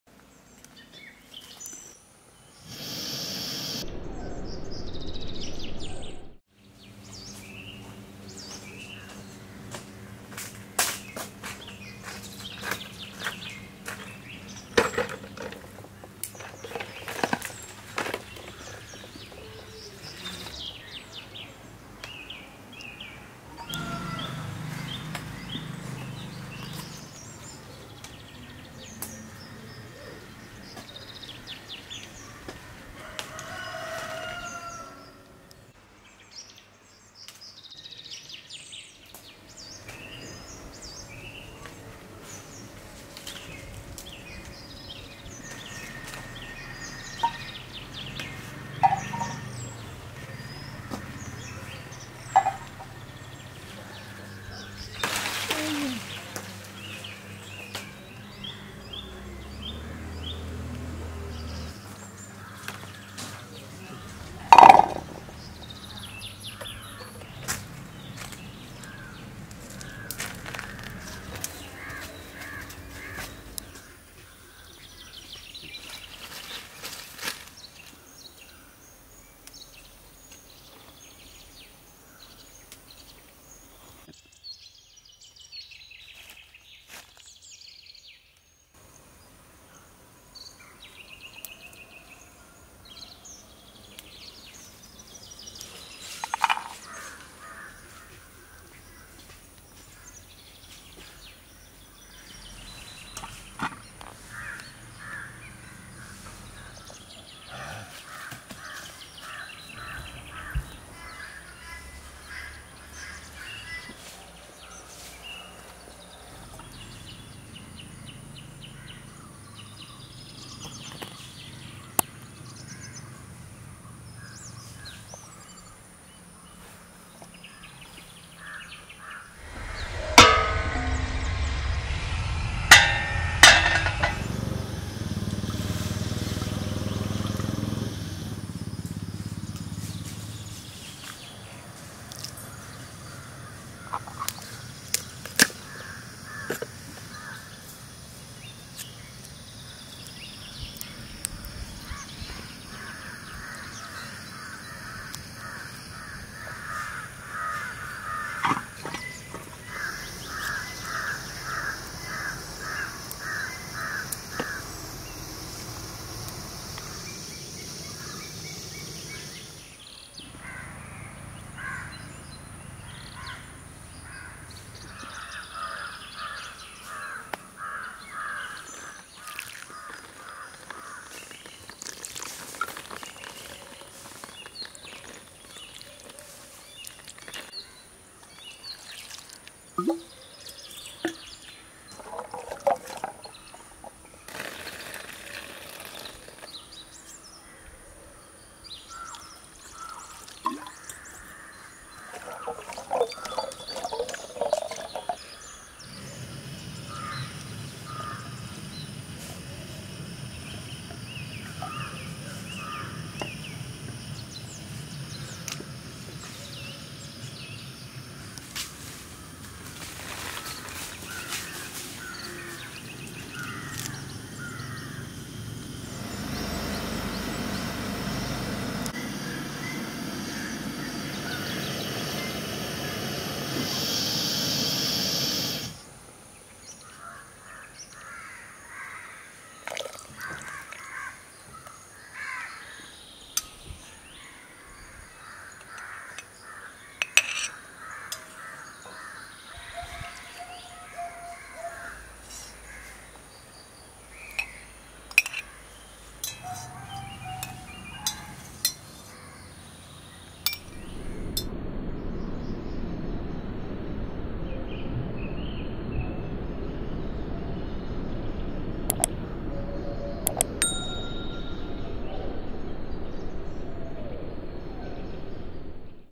🌶️ലൂബിക്ക ഉപ്പിലിട്ടത്🔥 Village Cooking Kerala (Outdoor) - LUBIKA | LUVIKA & KANTHARI Uppilittathu
Scram berry or Indian Prune & Bird's eye Chili in Brine

Ololika | Lololika | Luvika | Lubika - one of our favorite fruits which has several names. As we are all stuck at home, I decided to cook outdoor from our garden,  Luvika which we all love.

Lubika uppilittathu Ingredients:
Lubika = 1 cup
Garlic = 2 tbsp
Salt
Kanthari
Curry leaves

In our village(Kerala), Luvika is used to make pickles, chutney, wine and jam. My grandmother used to store it in brine for almost a year till the next season and occasionally she would make a chutney which we all loved. കഞ്ഞിയും  ലുവിക്ക ചമ്മന്തിയും കൂട്ടി ഒന്ന് കഴിച്ചു നോക്കു.

Wikipedia says - Flacourtia rukam is a species of flowering plant in the willow family, Salicaceae. It is native to China, India, and much of Southeast Asia, where it grows in forests. It is also cultivated for its edible fruit. Common names include rukam, governor's plum, Indian plum, and Indian prune.